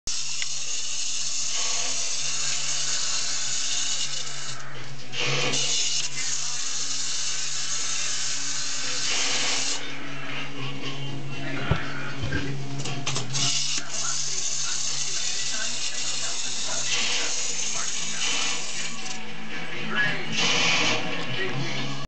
Snoopy Toy
Ein is scared of the snoopy toy!